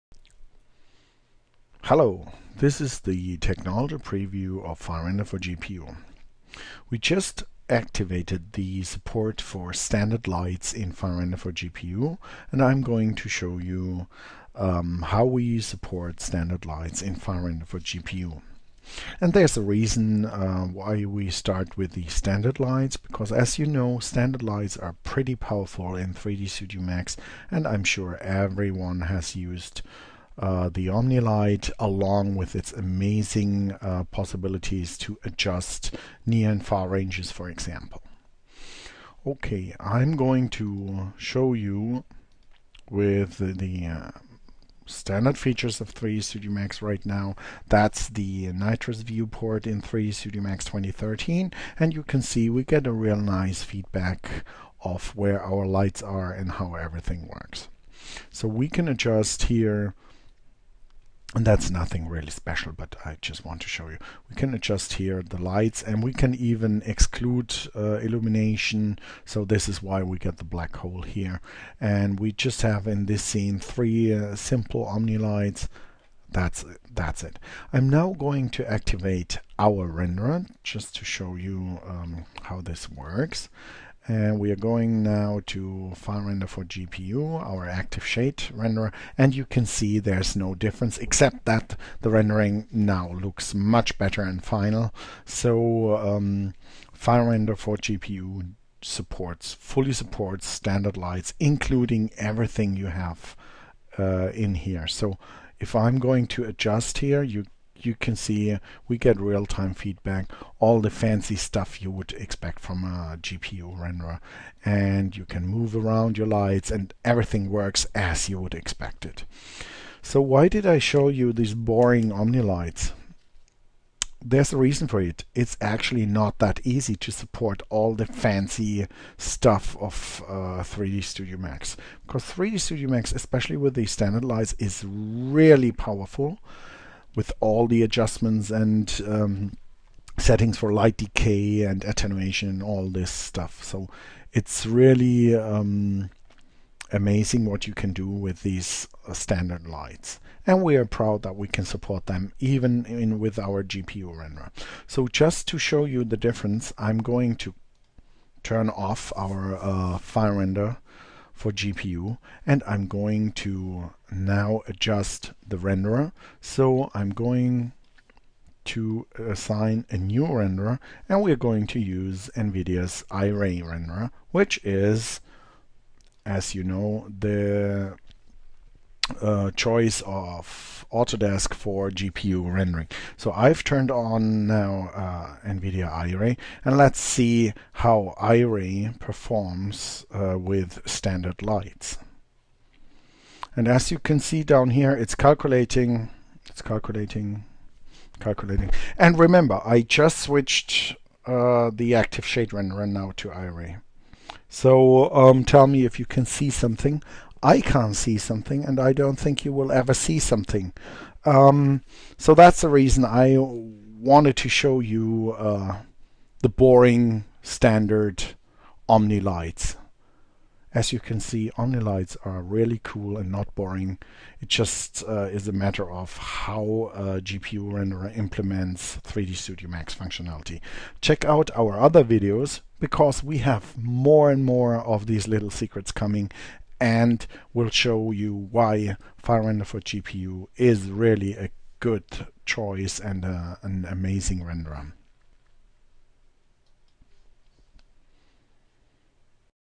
finalRender 4 GPU Standard Lights
Here in this video I'll play a little bit with standard lights. Keep in mind this is a tech demo and not the final Product! We just added support for the default lights as you will be able to see in this video. And just for the fun of iot I also compare it to NVIDIA iray.